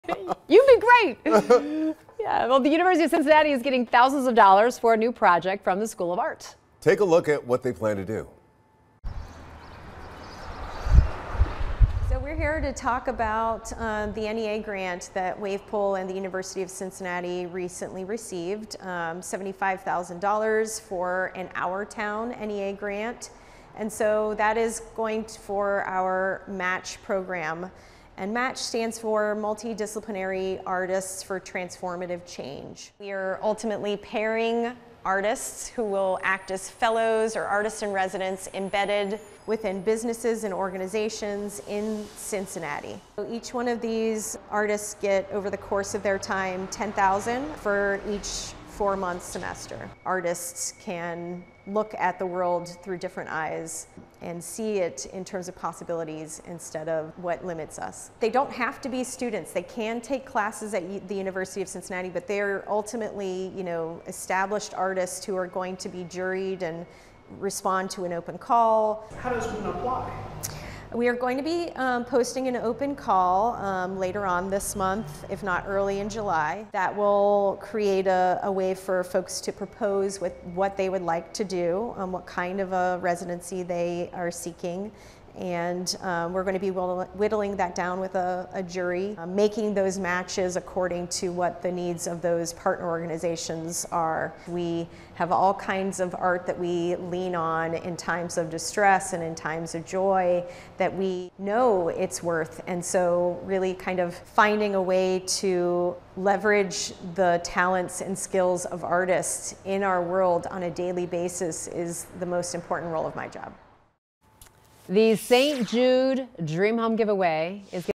University of Cincinnati gets federal grant for arts project
The University of Cincinnati is getting thousands of dollars from the National Endowment for the Arts for UC’s Multidisciplinary Artists for Transformative Change program.